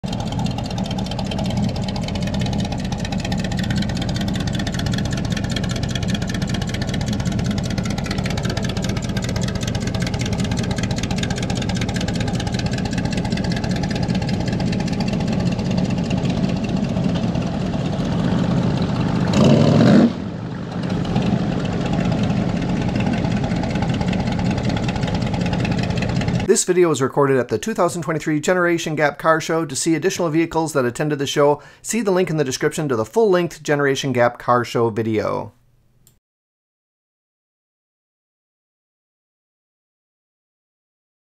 C3 Chevrolet Corvette - Generation Gap Car Show
Red C3 Chevrolet Corvette-at the Cold Steel Cruisers Generation Gap Car Show held at Route 65 Classics in Ham Lake Minnesota in 2023.

--Equipment Used on Vehicle Mundo --

📸--Equipment Used to create video--📸
GoPro Hero 11
GoPro Volta
GoPro Media Mod